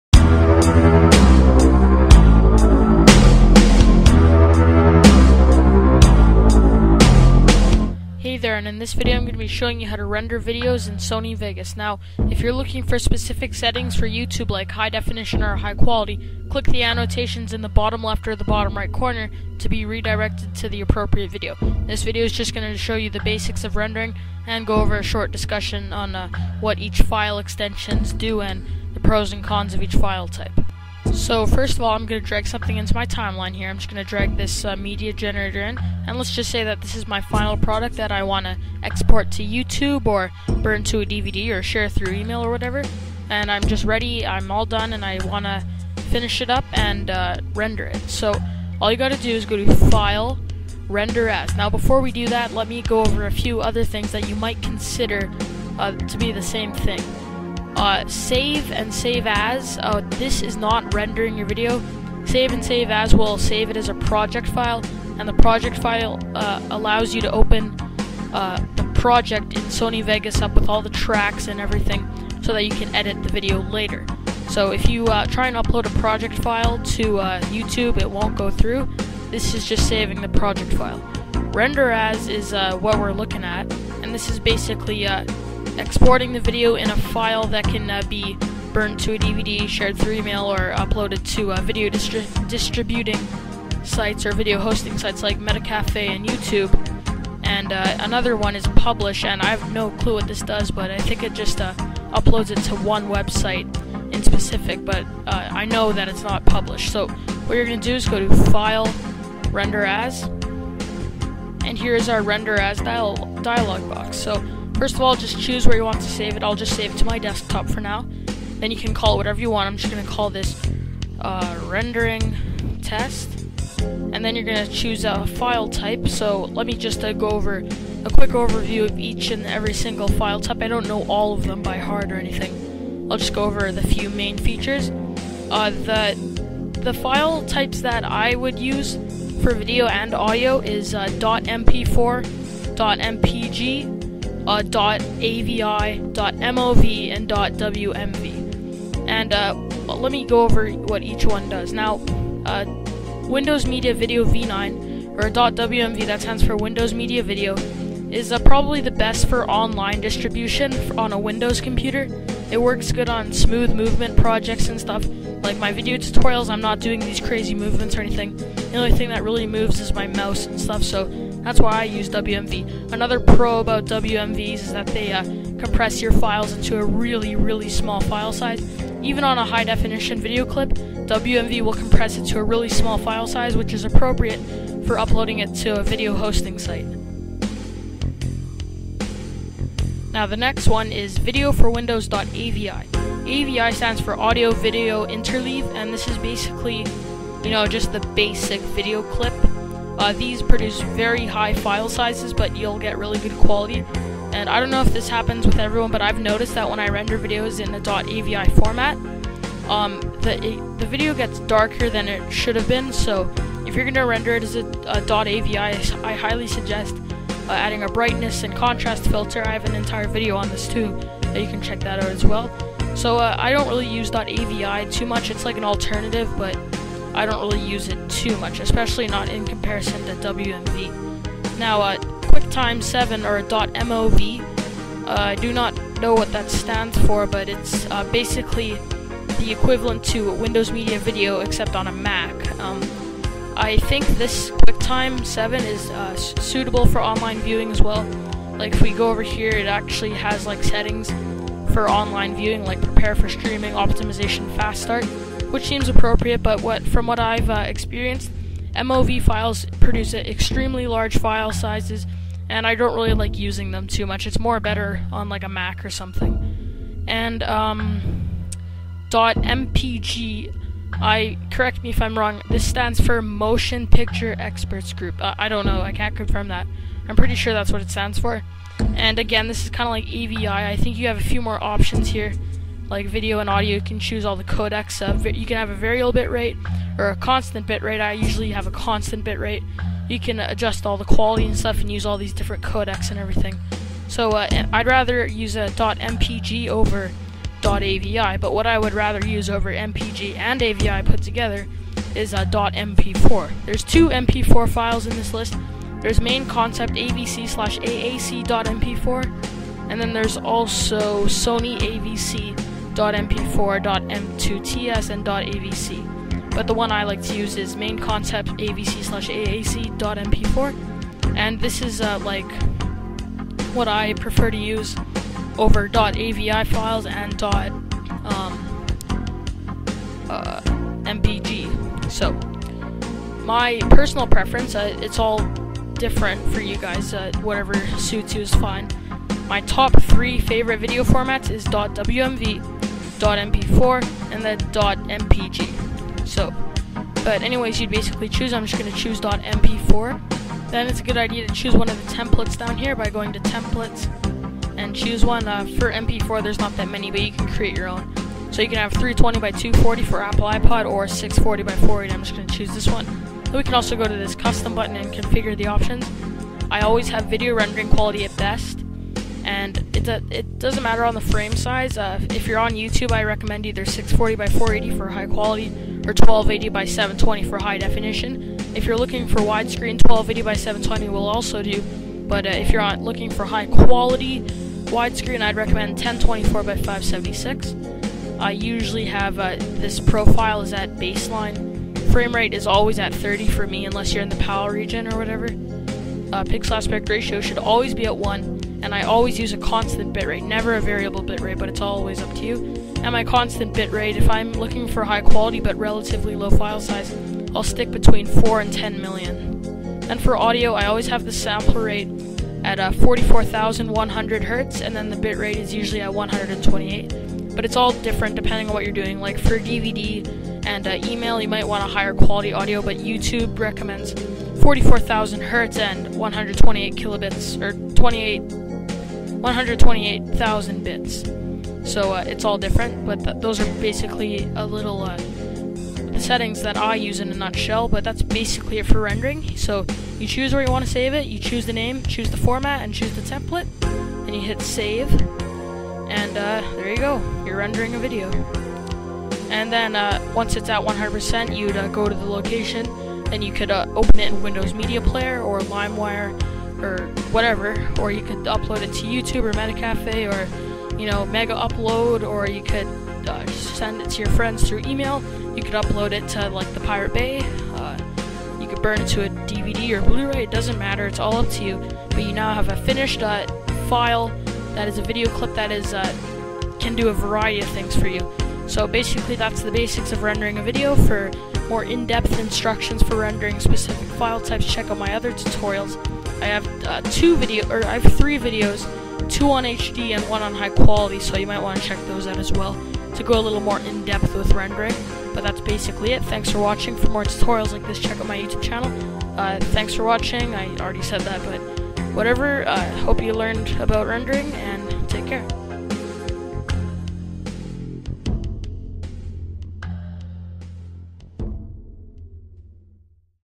Rendering Videos in Sony Vegas
This video will show you what rendering is, and how to do it.

-techtopia